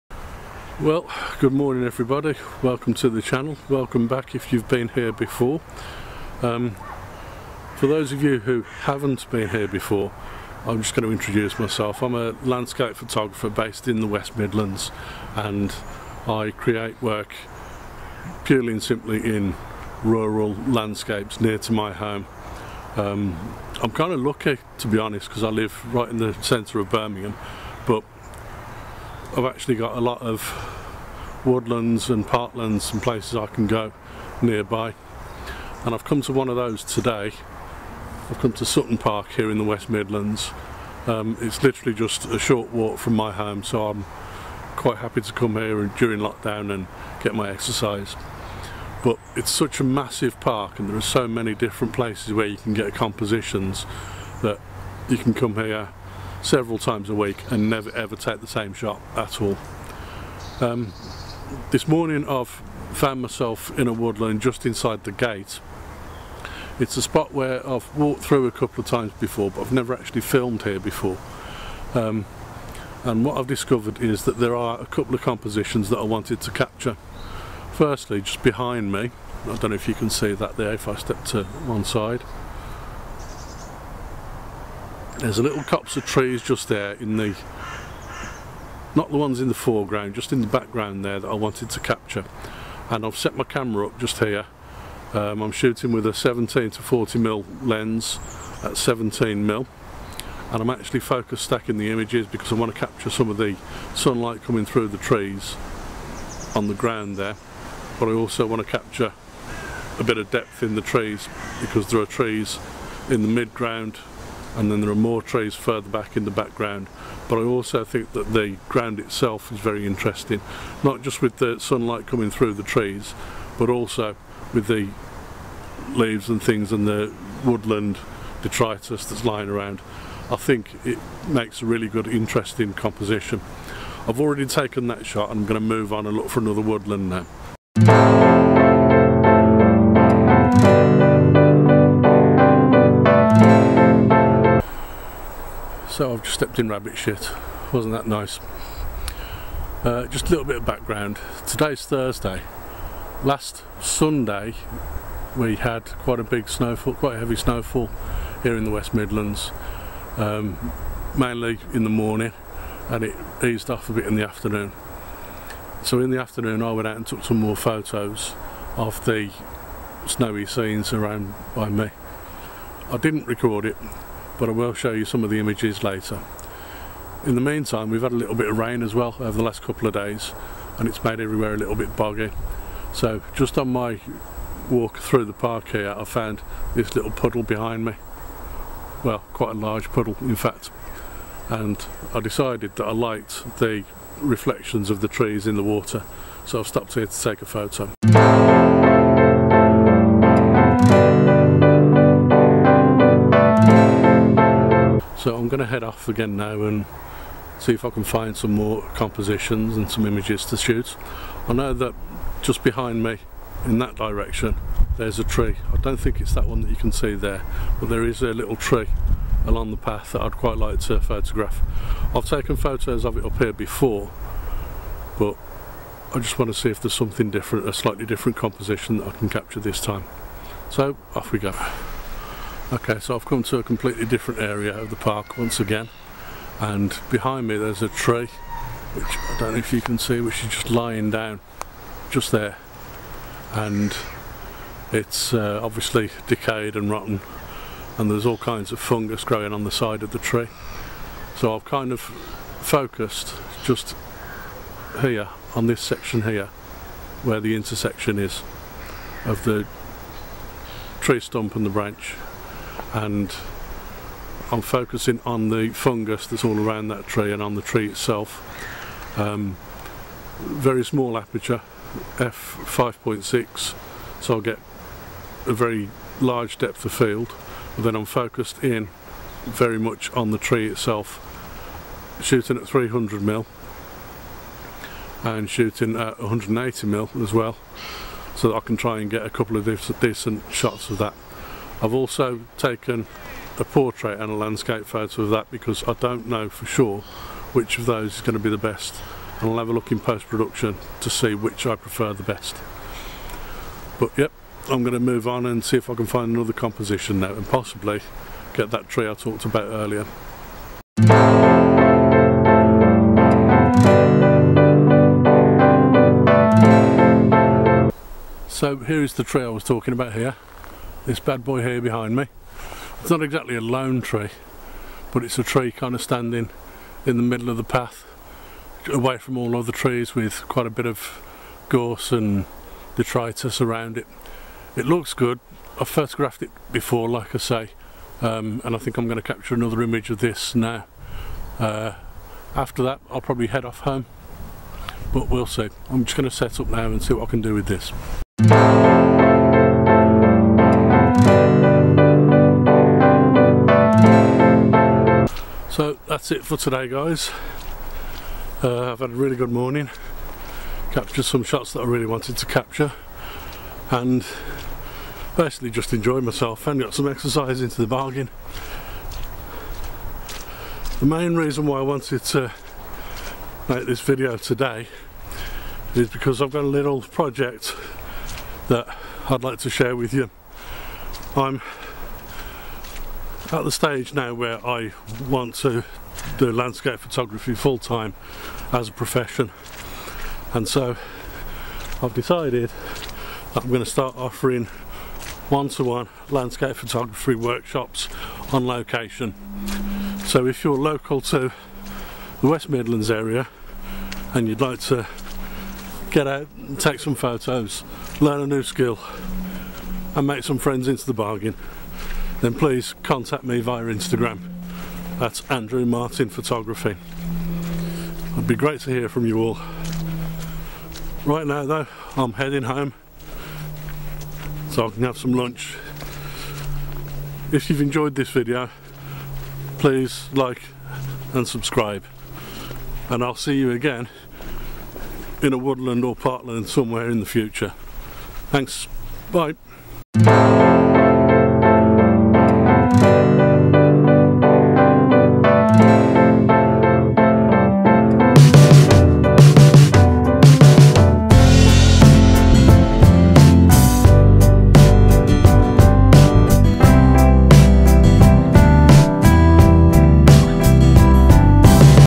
Landscape Photography and Stepping in Rabbit Shit!
A muddy walk in Sutton Park